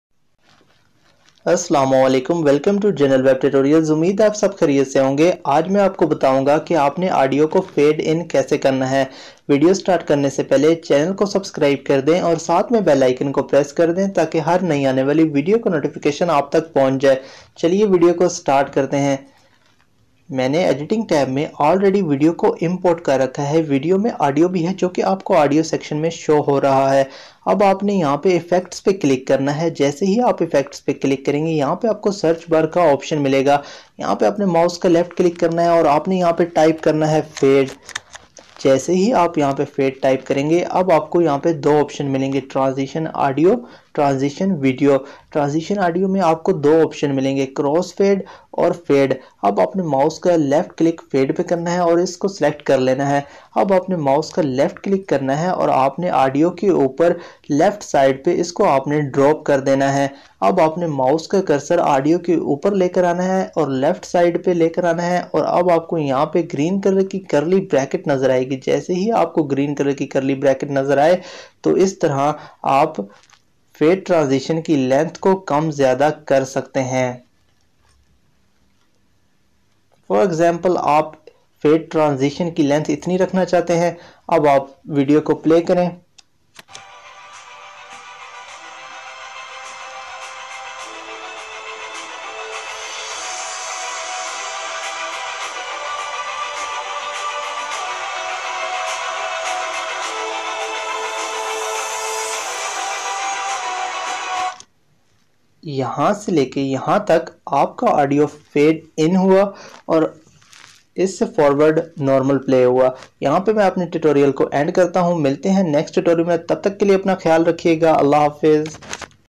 HitFilm Express : How To Fade In Audio ( Urdu / Hindi Tutorial )
In this video tutorial i will show you How To Fade In Audio . A tutorial on how to fade in audio in HitFilm Express . This works great to fade in music gain or other sound .